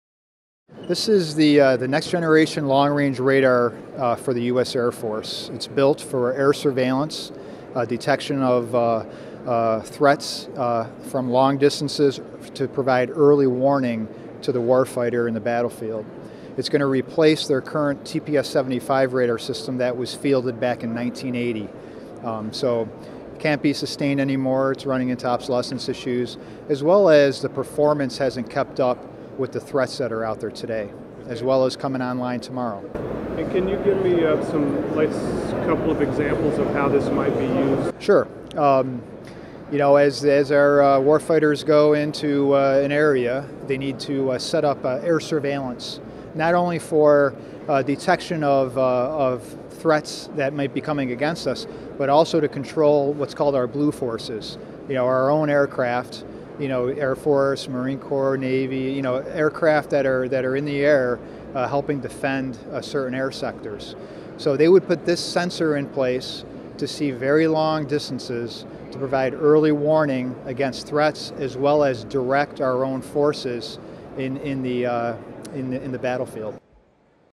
Lockheed's Next-Gen Radar is Ready to Roll
The recent Air & Space Conference near Washington, D.C., was an incredible display of some of the finest defense technologies in the world. Lockheed Martin (NYSE: LMT) showed off several products, including a giant three-dimensional long-range radar system -- which will be used by the Air Force for mobile, long-range surveillance and ballistic missile defense.

Motley Fool roving reporter Rex Moore was at the conference, and talked with Lockheed's director of ground-based surveillance radar, Mark Mekker. In this first of four installments, Mekker explains exactly what this 3D system is capable of.